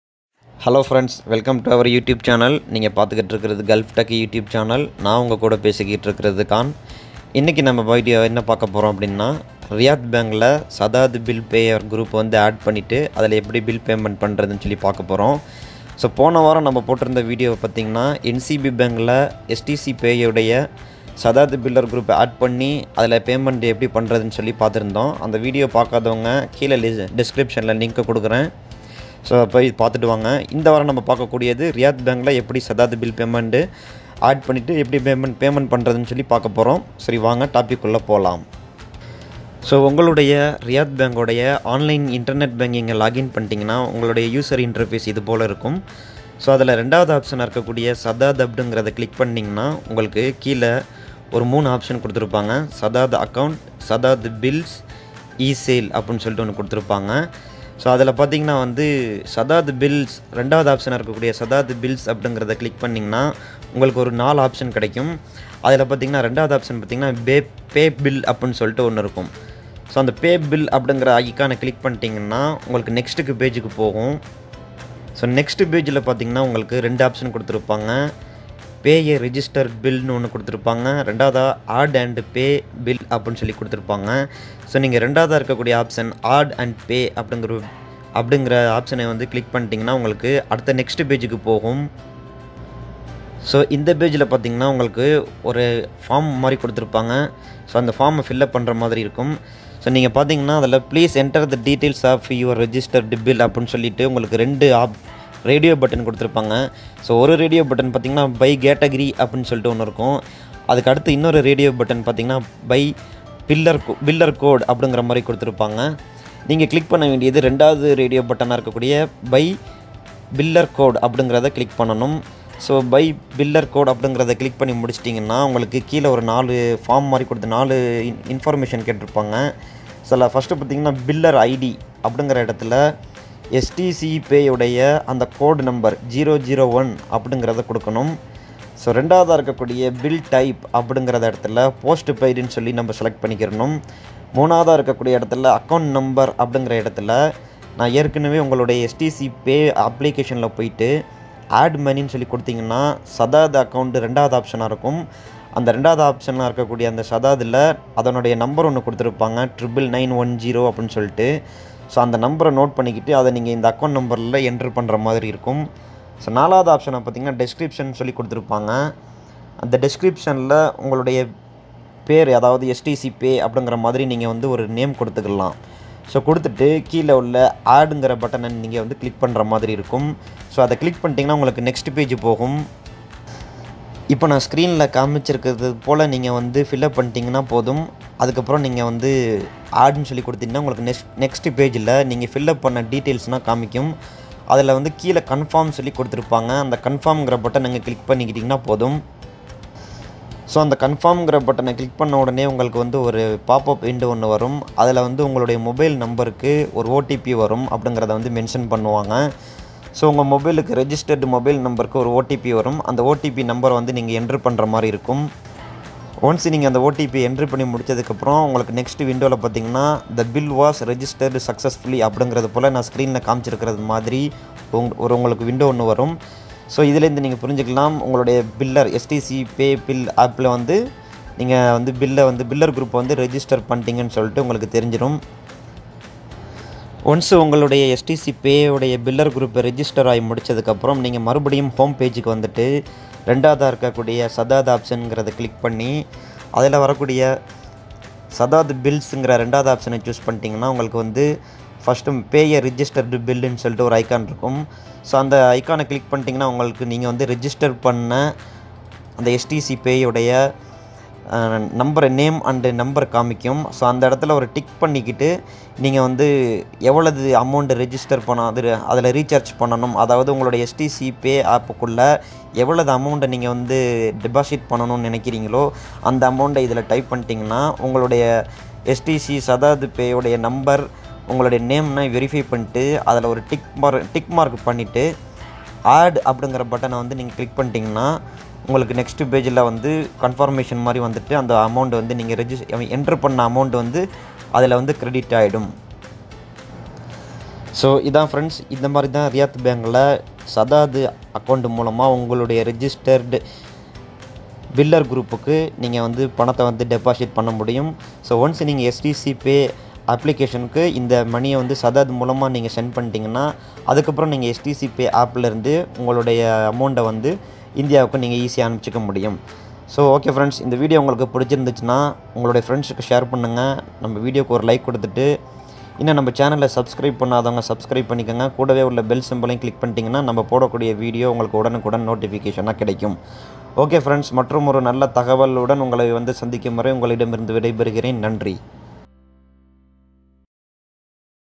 RIYAD_Bank‌ SADDAD_ACCOUNT STC_ Pay || How_to payment from Saddad to Stc Pay?
NCB மற்றும் RIYAD Bank ல் SADAD அக்கவுண்ட் உருவாக்குவது எப்படி? How to create SADAD Account in RIYAD BANK.

In this video, i am going to explain,  how to do the Payment for STC Pay through RIYAD Bank SADAD Account. Please watch the Video Until end. So that you will not get confuse. And If you have any doubts, please let me know in Comments Section below...